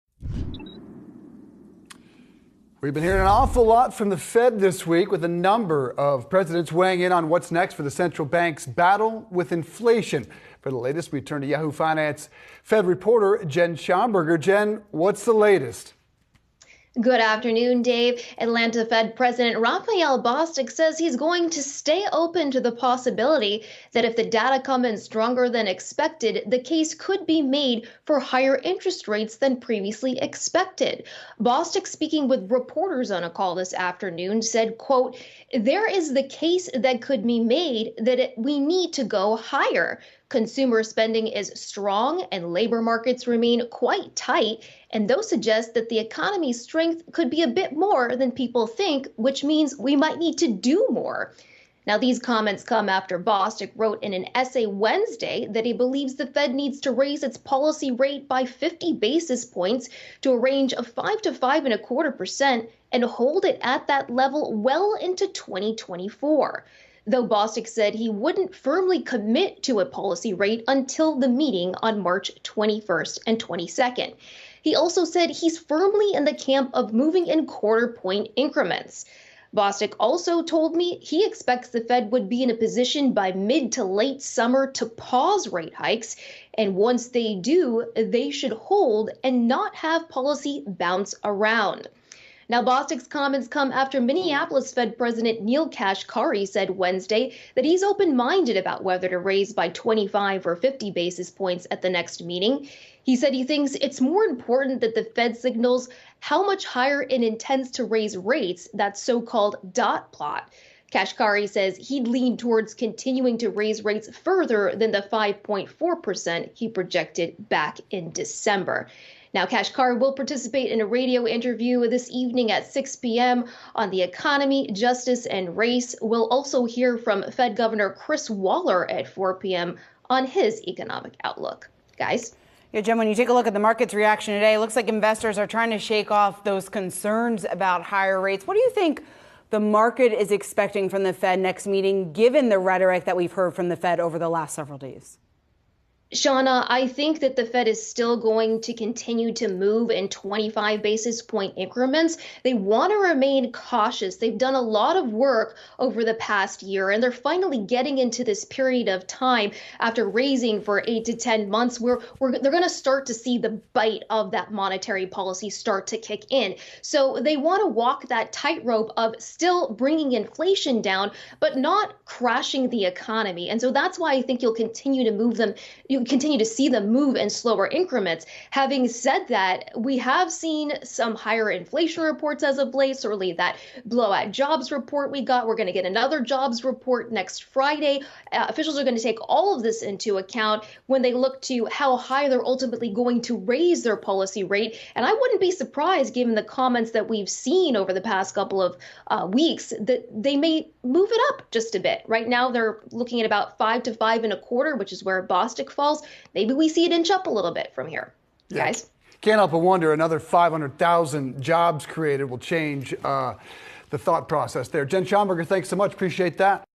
Fed officials remain ‘open-minded’ on future rate hikes
Yahoo Finance's Jennifer Schonberger joins the Live show to discuss recent statements by Fed officials on interest rate hikes.